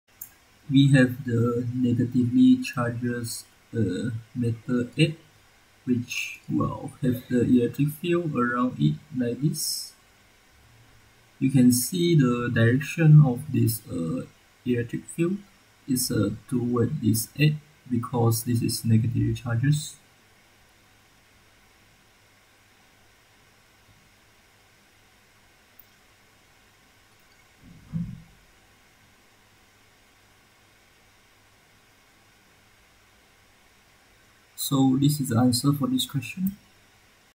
Chapter 22 | Problem 12 | Physics for Scientists and Engineers 4e (Giancoli) Solution
Draw the electric field lines around a negatively charged
metal egg.
Chapter 22 | Problem | Physics for Scientists and Engineers 4e (Giancoli) Solution